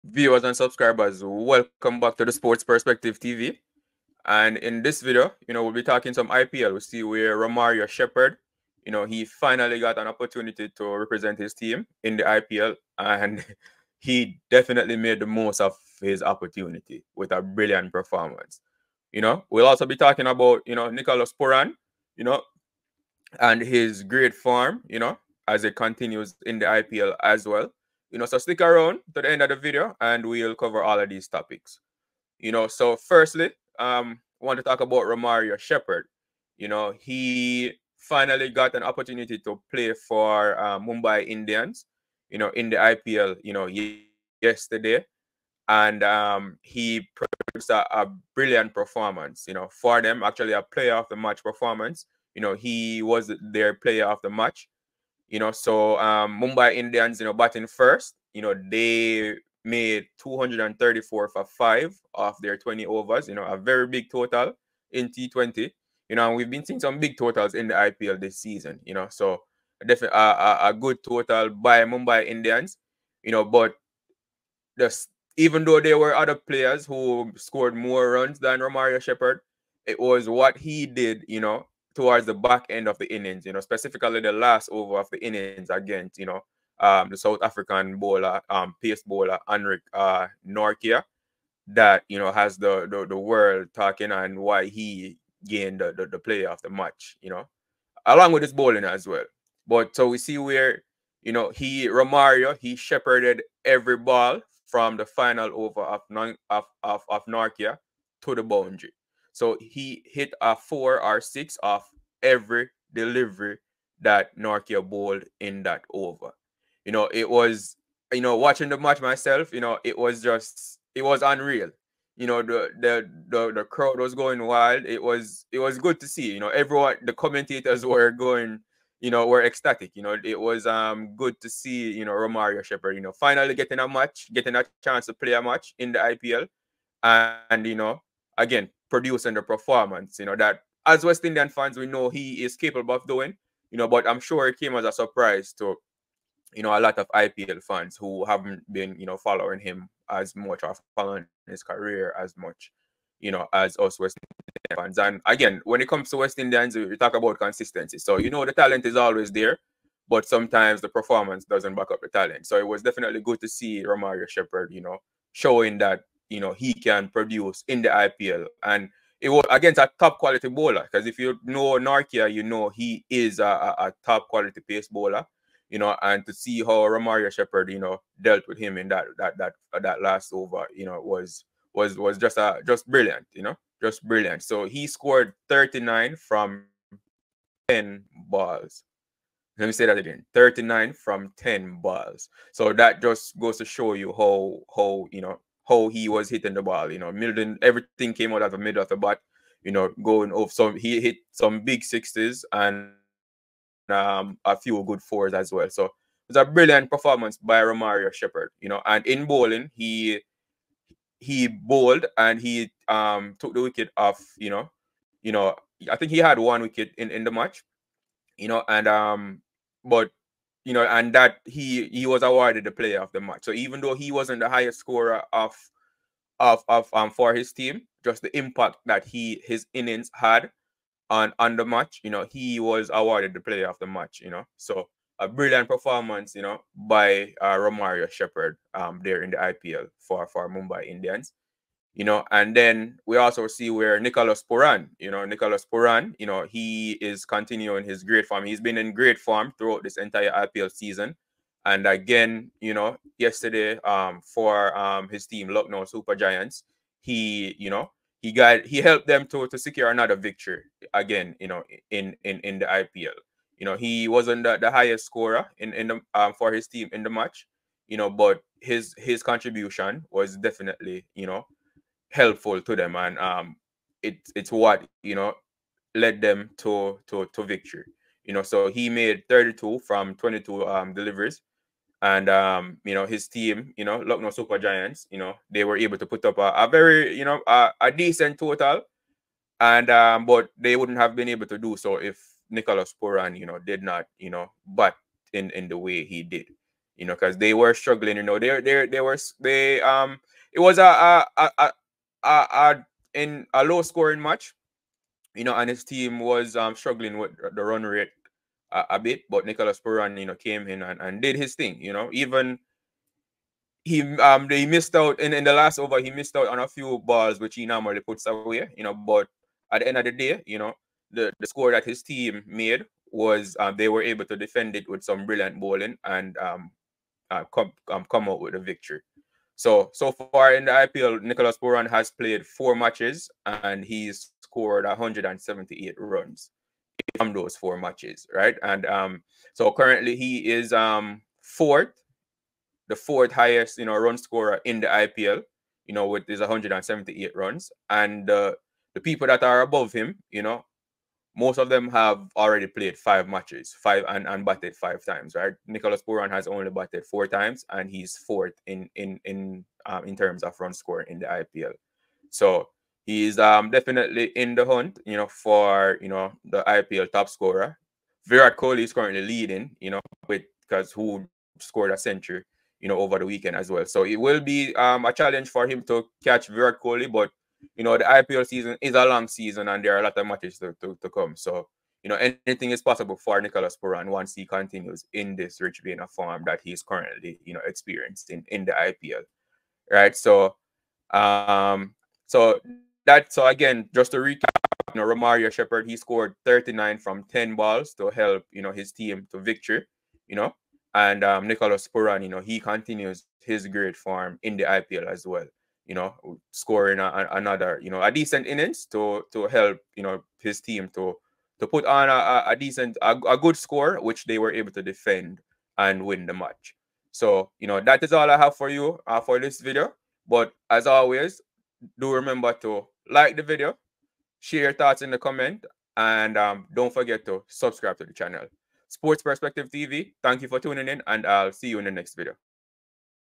Shepherd Hits Boundary From Every Ball in Nortje Over | Player of Match | Pooran Good Form Continues
Shepherd Hits Boundary From Every Ball in Nortje Over | Player of Match | Pooran Good Form Continues . Nicholas Pooran batting | IPL match |IPl matches